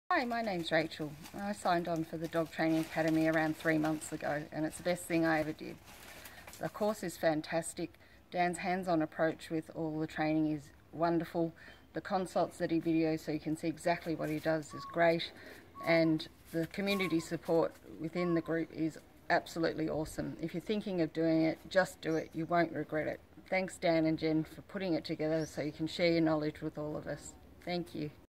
Dog Trainer Academy Testimonial: Rachel
- enquire about the Dog Trainer Academy now!
Witness how Doggy Dan's academy is changing people's lives, helping them find TRUE PASSION and SUCCESS as Professional Certified Dog Trainers.
Or, get started with the EASIEST and FASTEST program for transforming your dog